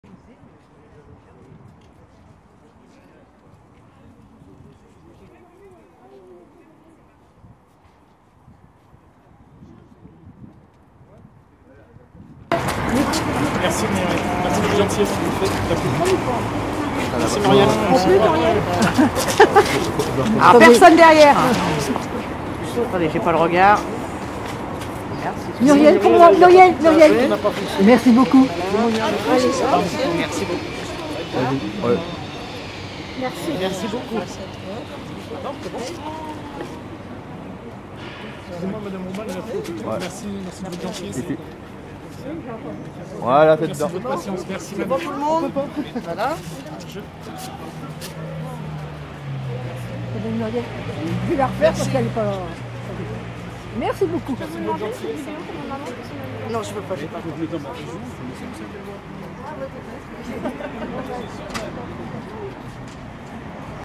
Muriel Robin arrives at Vivement Dimanche in Paris
Muriel Robin strikes the pose fan the fans at her arrival at Vivement Dimanche Tv Show in Paris
Paris, France on Monday December 10, 2018